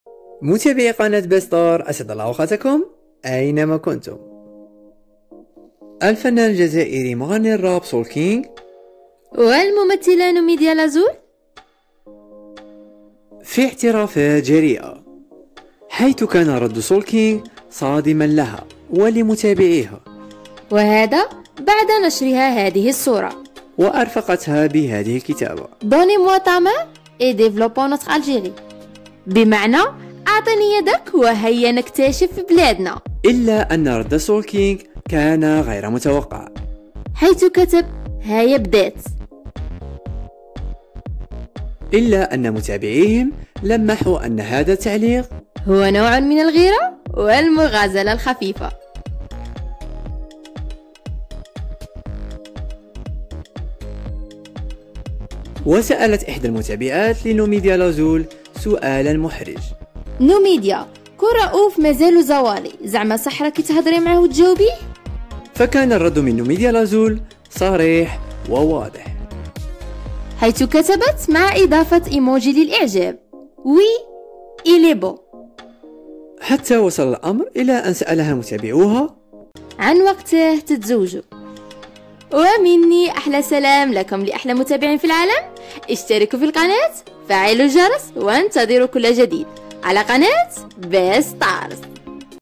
نوميديا تعترف💞و سولكينغ يصدمها ب ردة فعل مفاجئة😱 و غير متوقعة 😱
نوميديا تعترف و سولكينغ يصدمها بردة فعل مفاجئة و غير متوقعة

سولكينغ يغار على نوميديا لزول و يشعل الإنستغرام بتعليقه على فديو نوميديا الاخير"دوك نصرعك"😨

  نوميديا لزول تعترف ل سولكينغ بأنه المفضل لقلبها 💞😍 و الانستغرام يشتعل بعد هذا  فديو